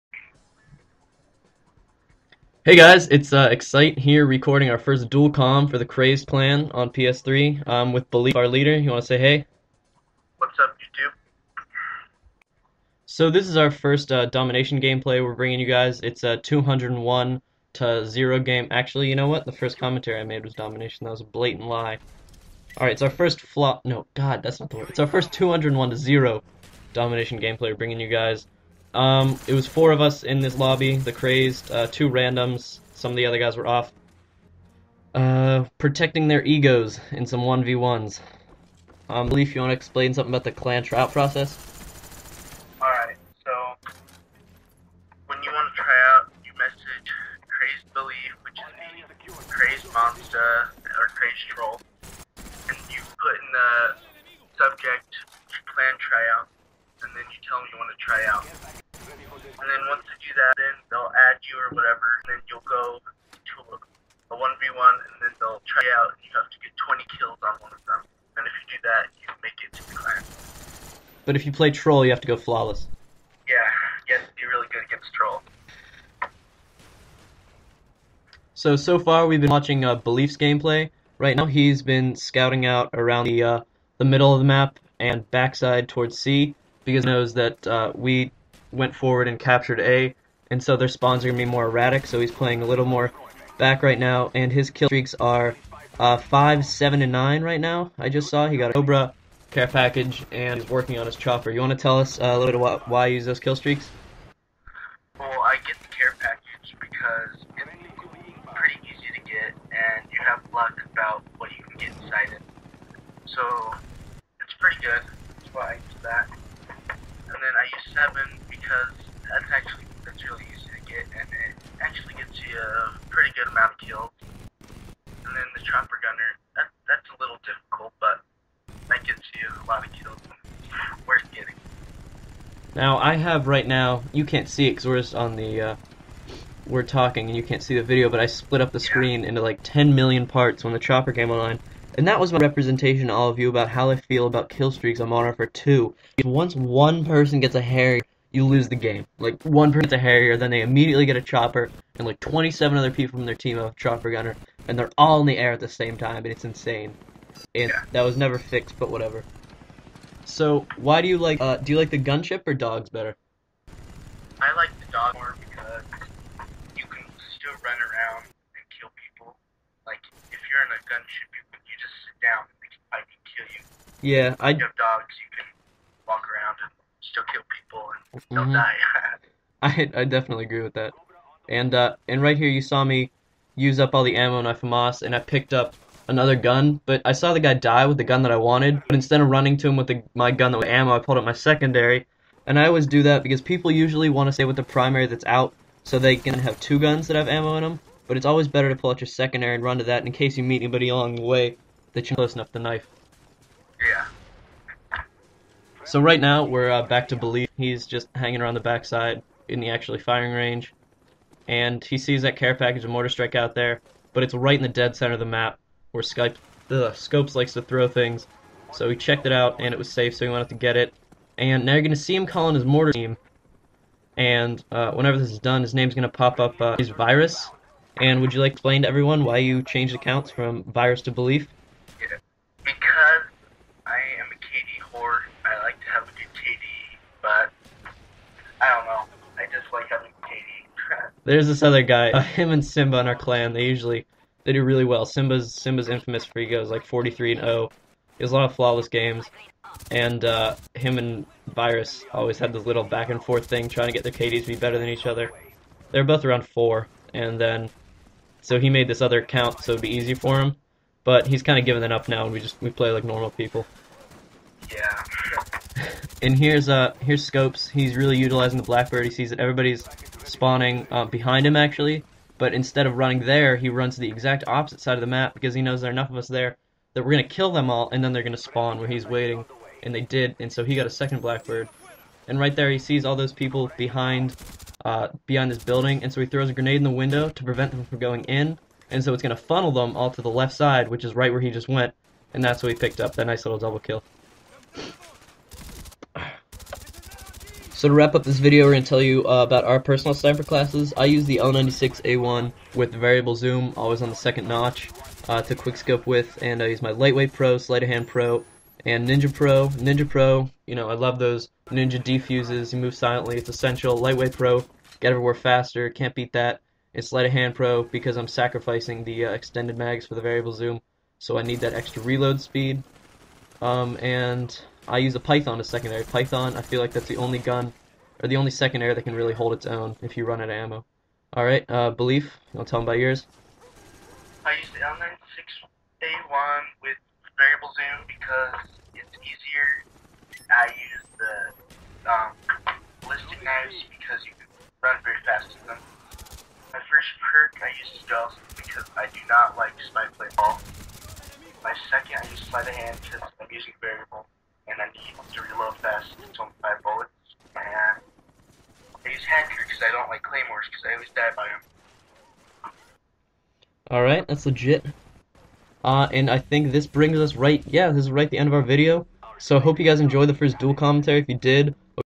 201-0 Domination Dual Commentary
Hey guys, this is the first dual commentary we have made, I (excite) had belief on the phone for this one and i plan to get more with him and try to have everybody that I can in a dual commentary with me soon, I will keep posting quality content and god news xCrAZzeD_Zap is editing for us so he will be making clan montages soon! And i personally know i have a little Mw2 one coming at you guys shortly.
~ExcitE